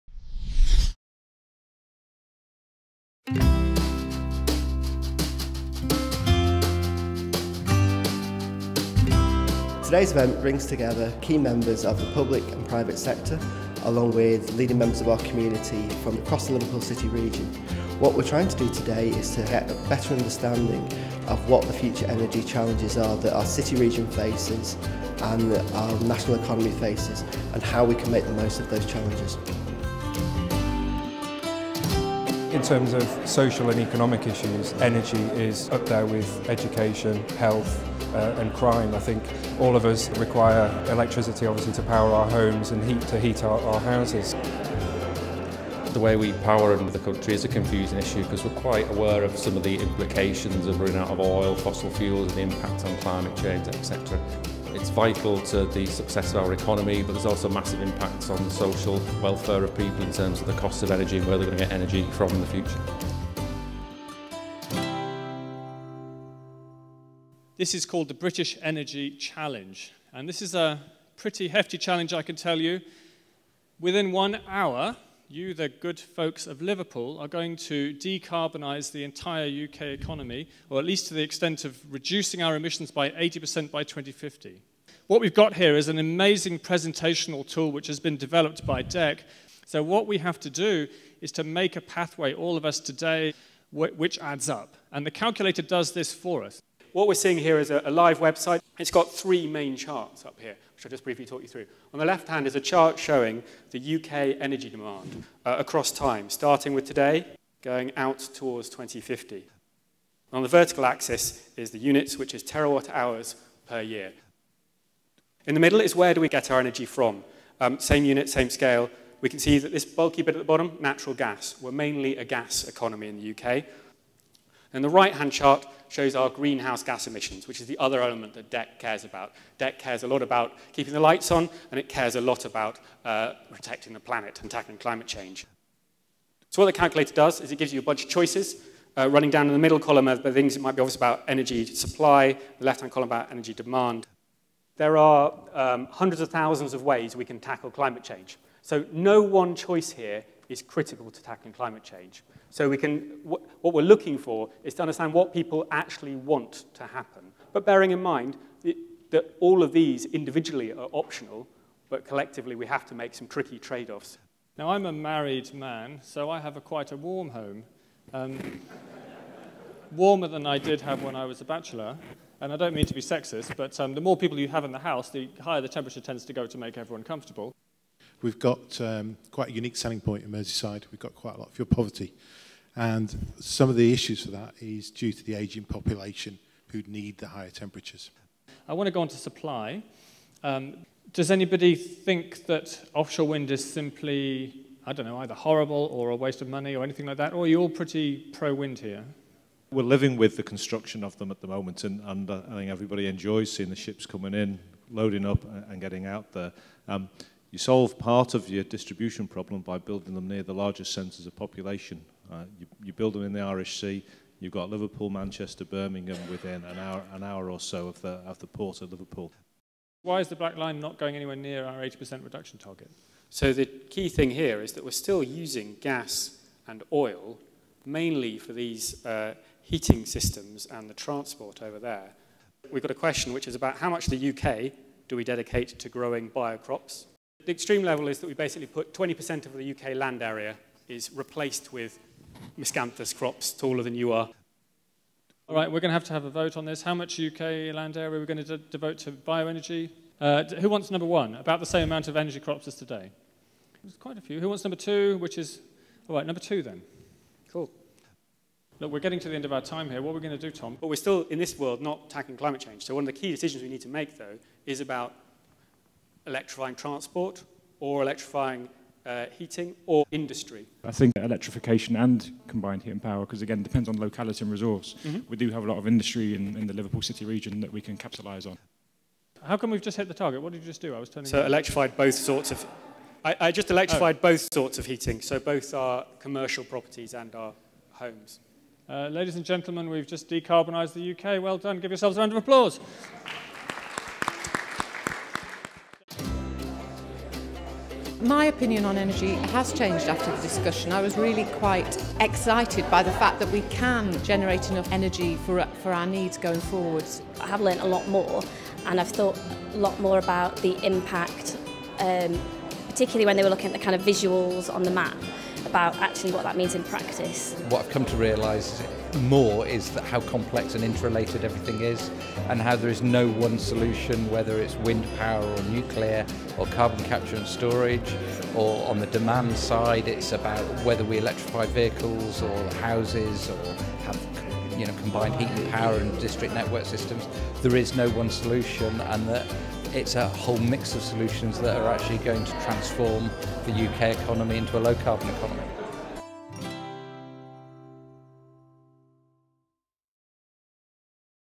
The British Energy Challenge at Liverpool
Find out what happened when the first British Energy Challenge roadshow visited Liverpool. The purpose of the event? In just one hour the audience had to find a way to power the country while cutting carbon emissions.